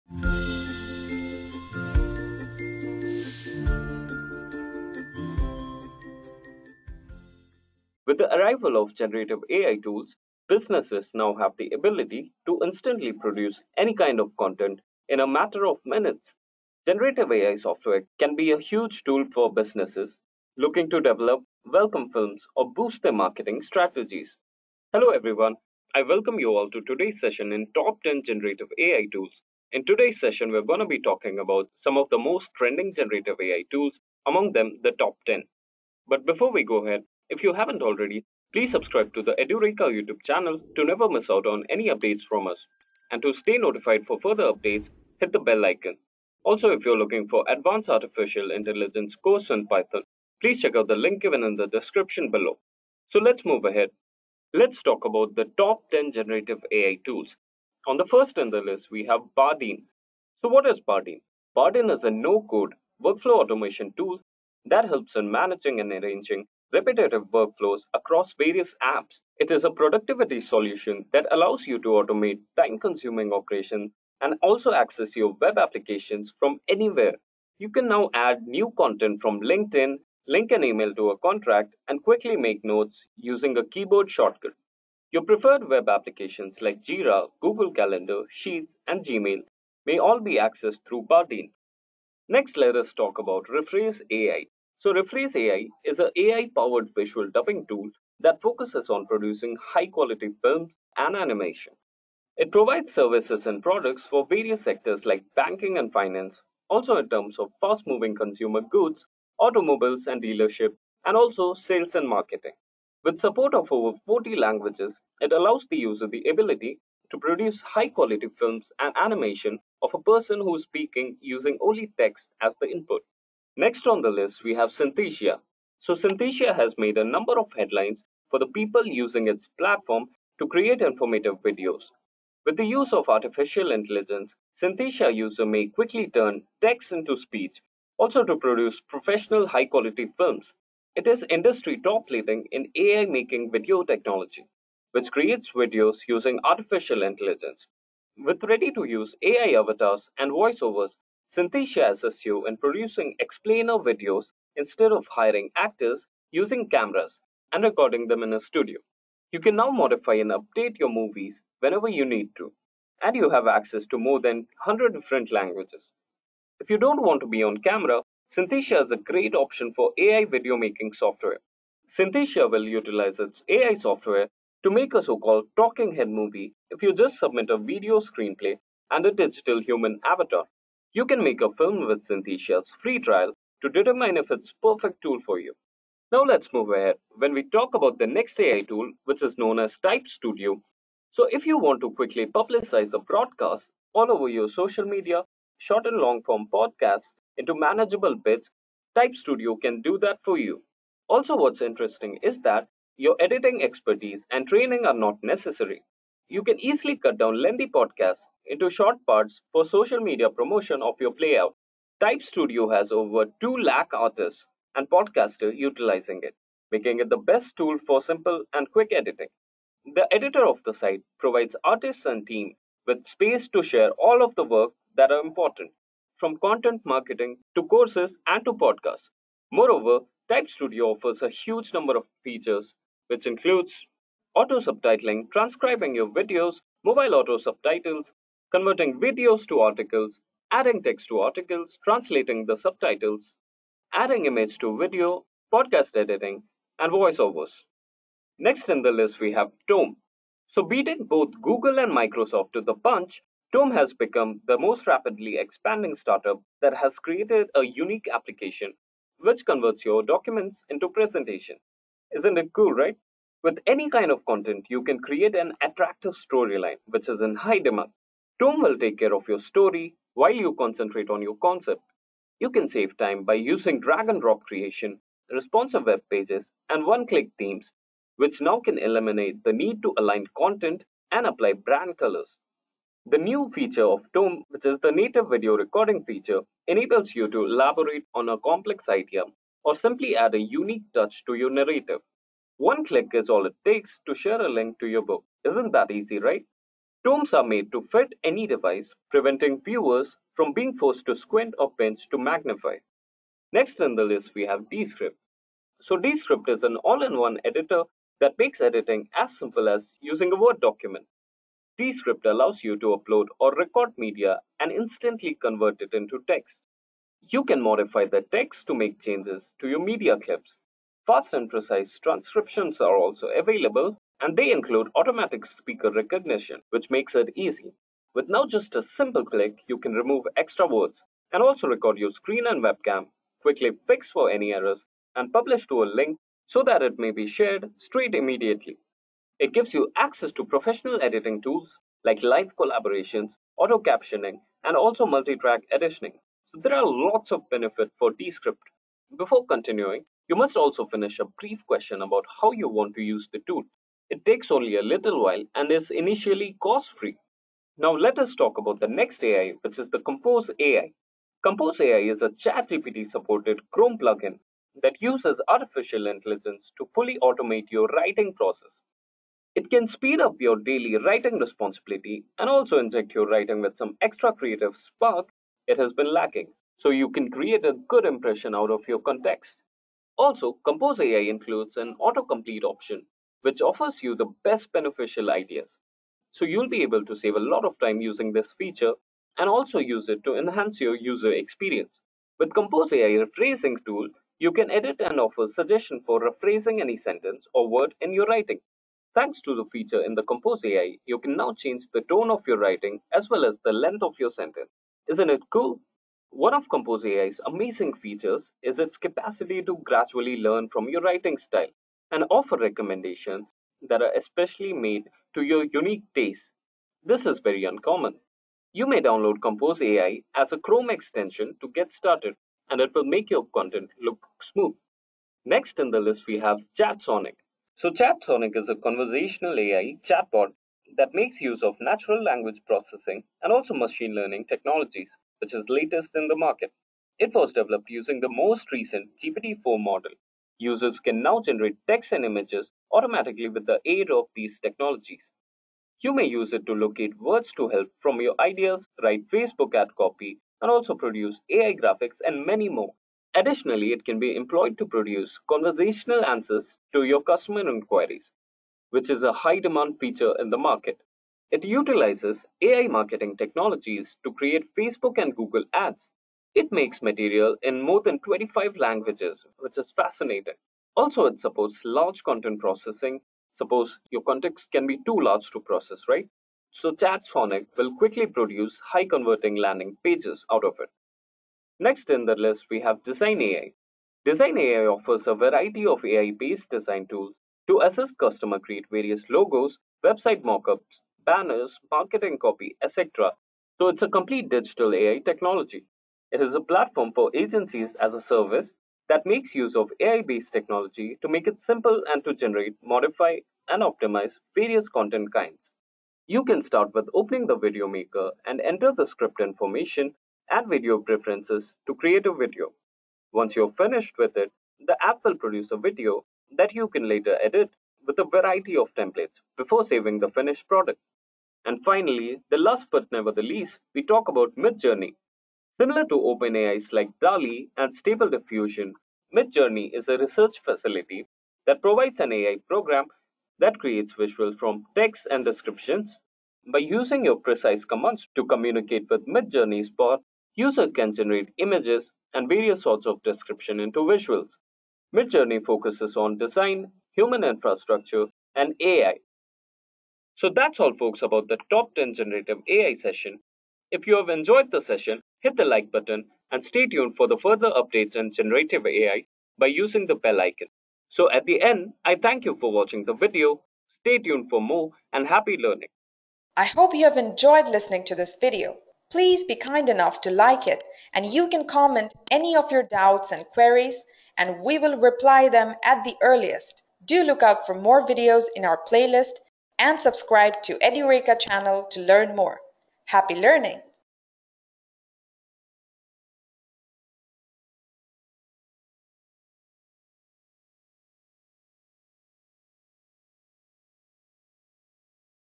Top 10 Generative AI Tools | AI Edureka
🔥𝐄𝐝𝐮𝐫𝐞𝐤𝐚 𝐏𝐫𝐨𝐦𝐩𝐭 𝐄𝐧𝐠𝐢𝐧𝐞𝐞𝐫𝐢𝐧𝐠 𝐰𝐢𝐭𝐡 𝐆𝐞𝐧𝐞𝐫𝐚𝐭𝐢𝐯𝐞 𝐀𝐈 𝐂𝐨𝐮𝐫𝐬𝐞 (Use code "𝐘𝐎𝐔𝐓𝐔𝐁𝐄𝟐𝟎")
In this Edureka video on “ Top 10 Generative AI tools”, you will learn about the top 10 Generative AI tools, I aim to show you what are the most trending Generative AI tools and how it is going to change our world. What do you think about these new AI Tools?